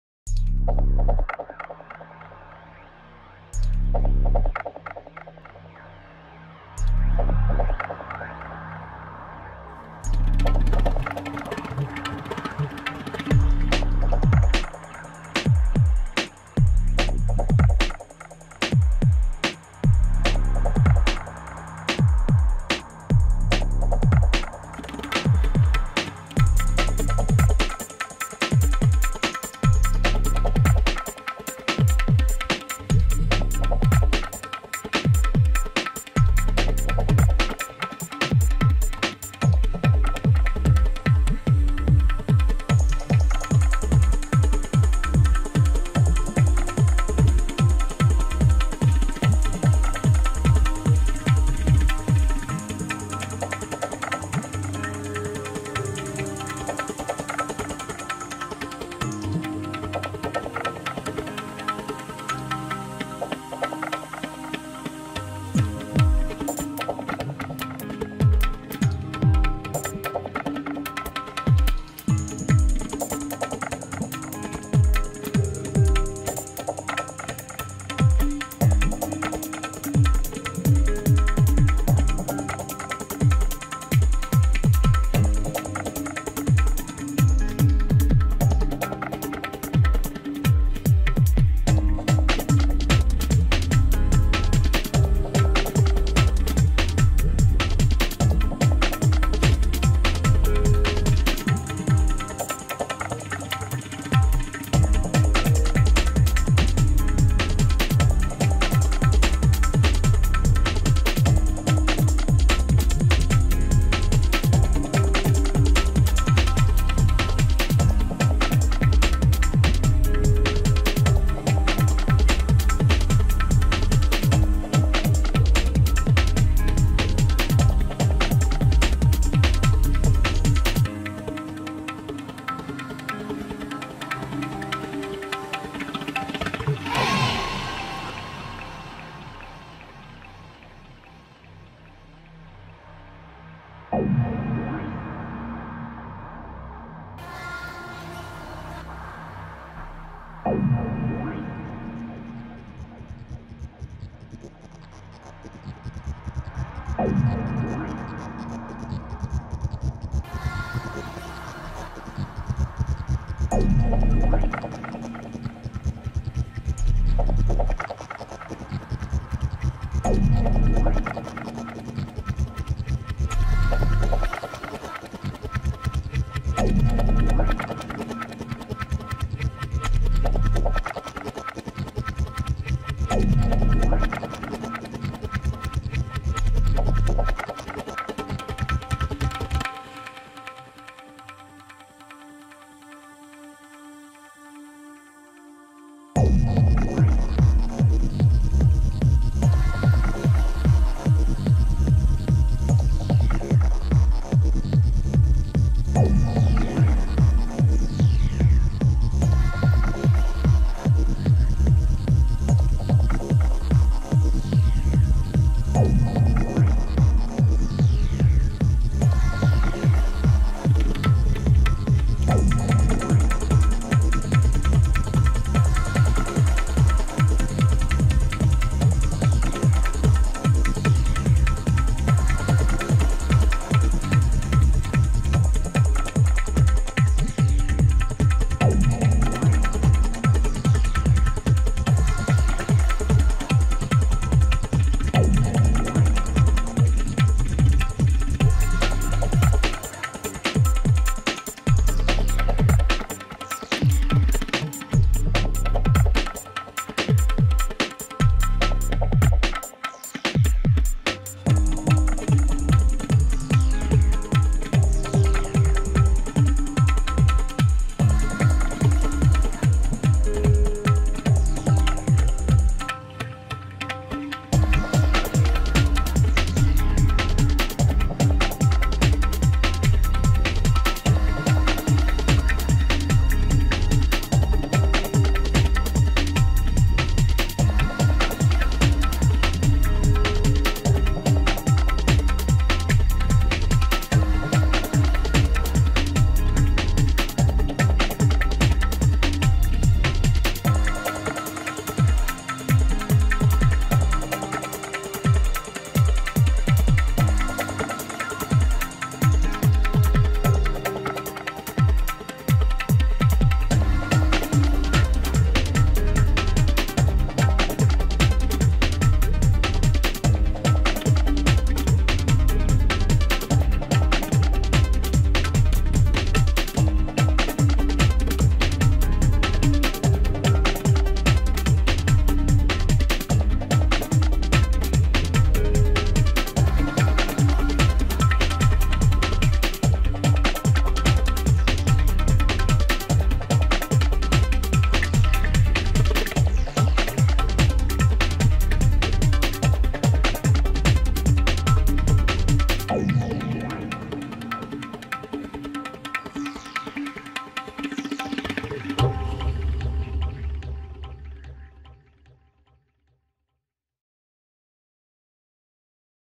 Goats Riseing 8
Trance music with psychedelic film clip.